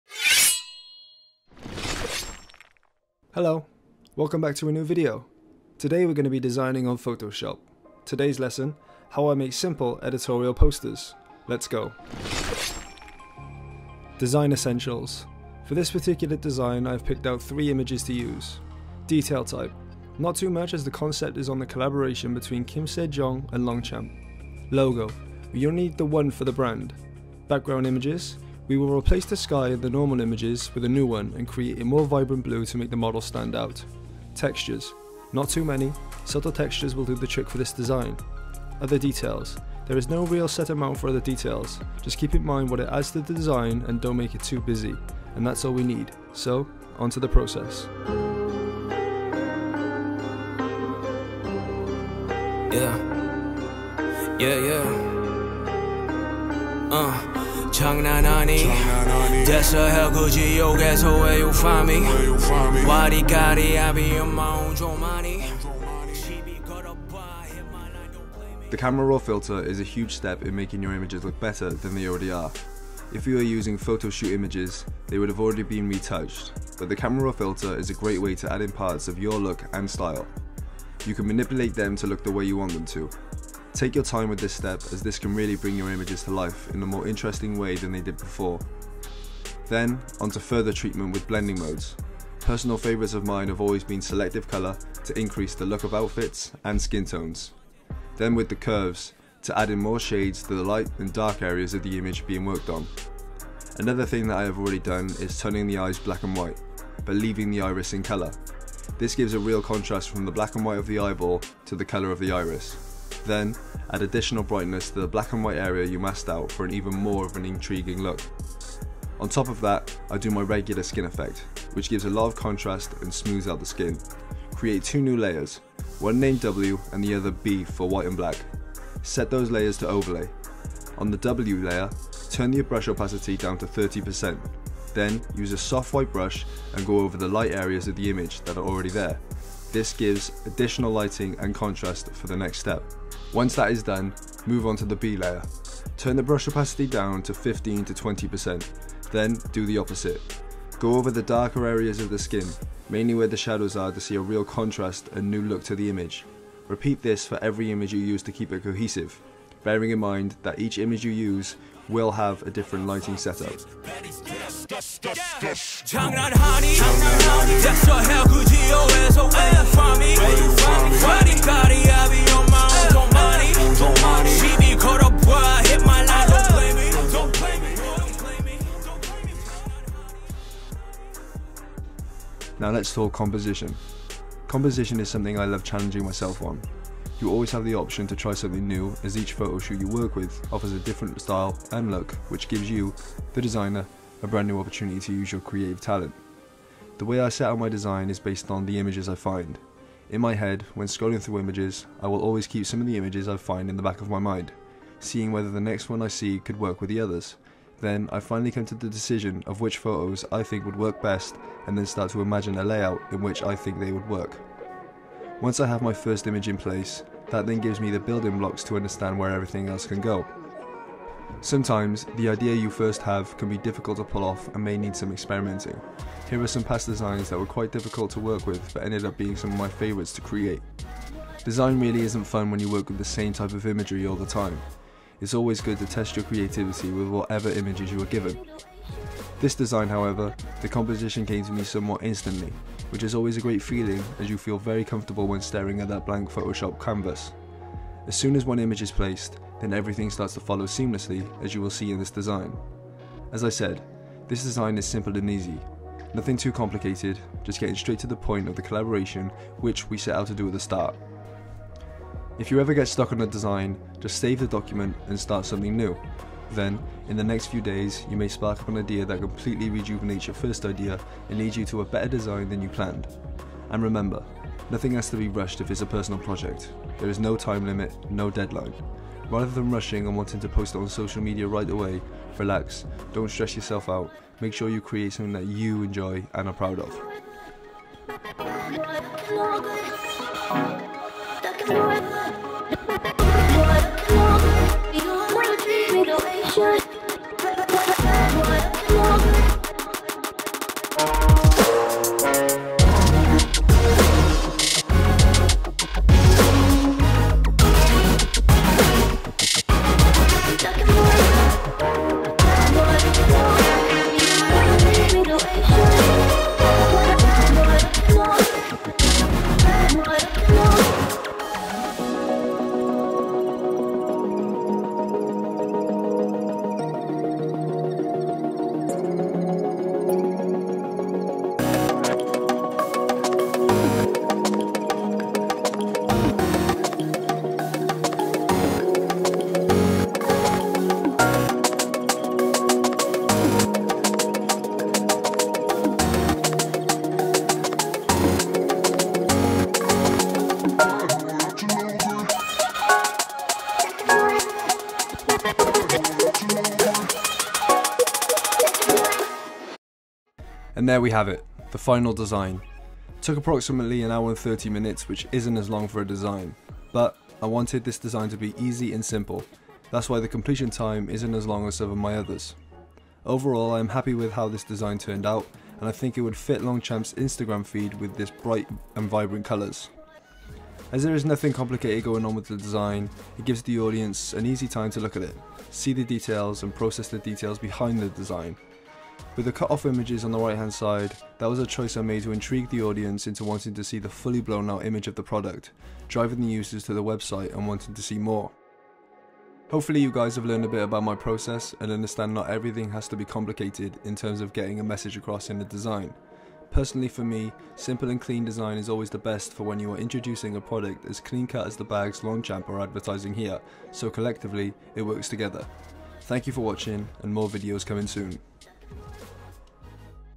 HOW I CREATE EDITORIAL POSTERS ON PHOTOSHOP TUTORIAL! ft. KIM SEJEONG
Hello, I'm Rhys, a Graphic Designer from Wales. In this video, I will be showing you my technique and process to create an Editorial Poster.

Hopefully you enjoy the video and learn something new whilst you're at it.